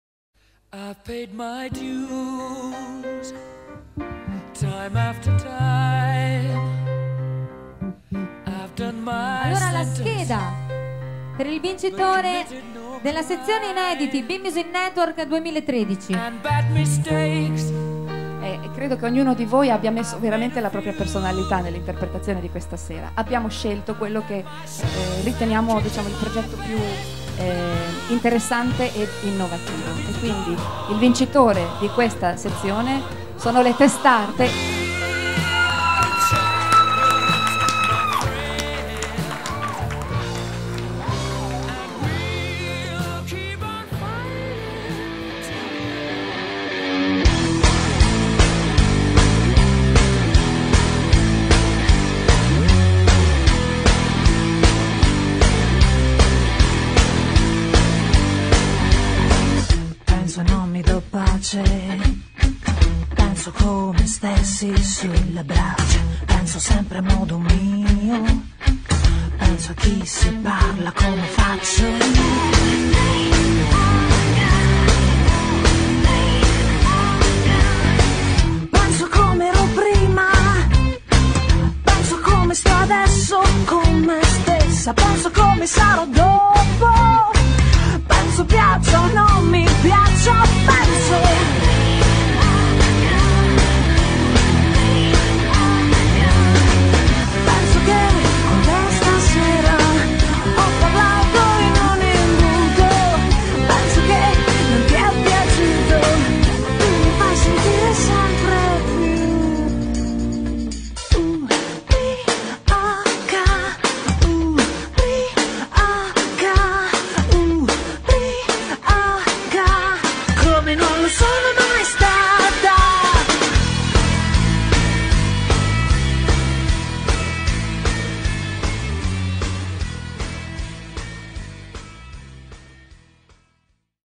TestHarde - 1' parte - la vittoria
... e il concorso Bim Music Network di Bellaria Igea Marina è vinto da... le TestHarde !!!
Bravissime davvero!!!